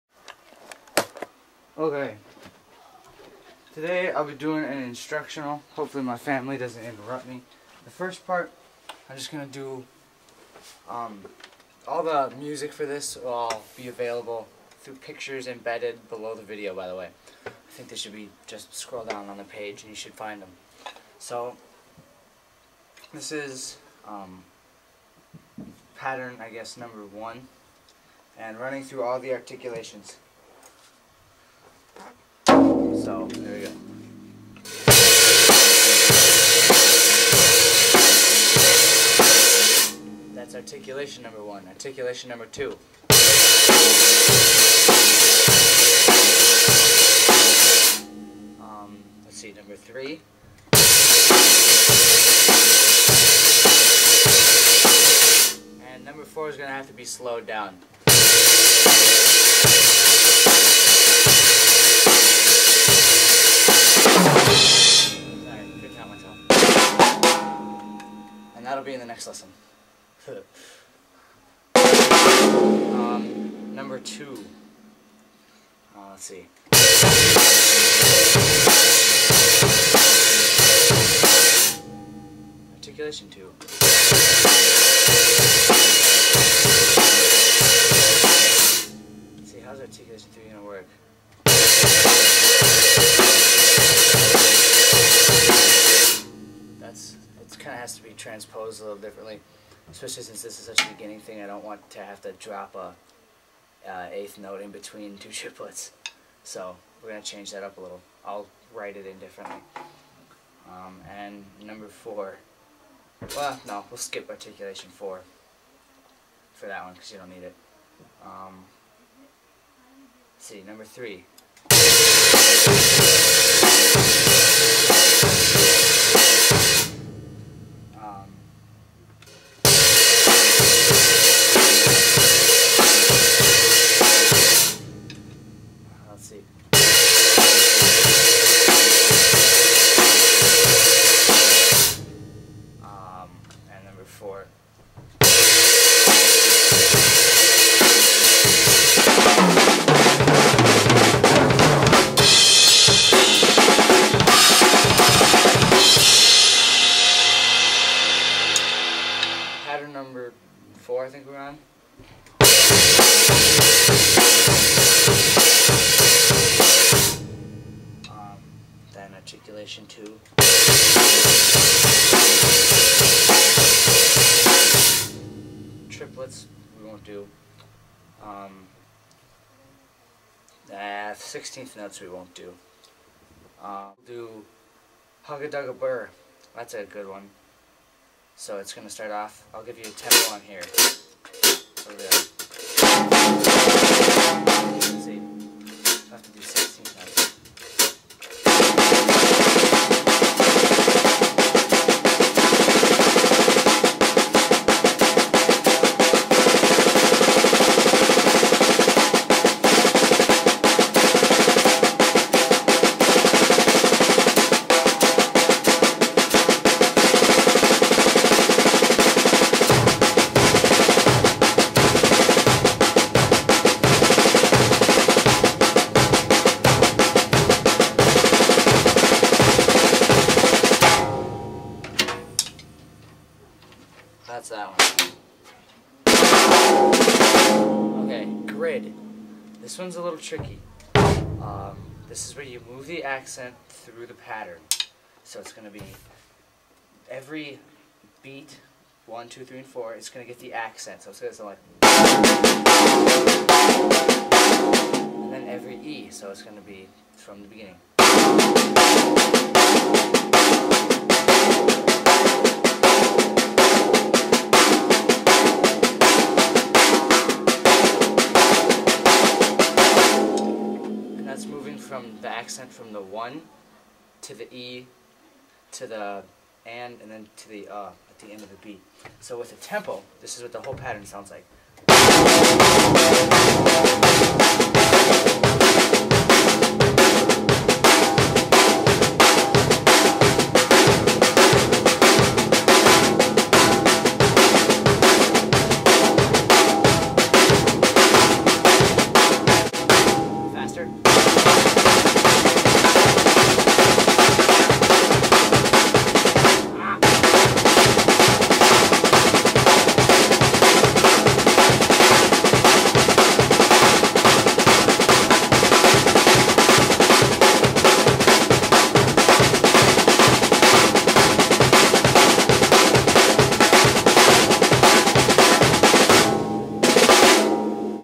Drum Lesson With Eddie!
me giving u all a little advice on playing.

please comment if u have an account.

my myspace is

www dot myspace dot com slash edrsmyspace

there u go.